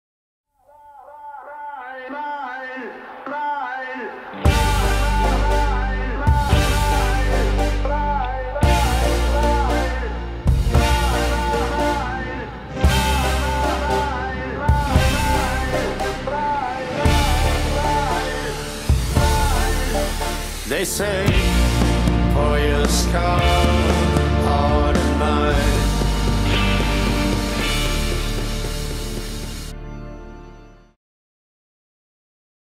Trailer 2017 "Le Père Noël est-il un Rocker ?" 23ème
Trailer court officiel de la 23ème édition du festival de musique solidaire "Le Père Noël est-il un Rocker ?" qui aura lieu à Lille du 14 au 18 novembre !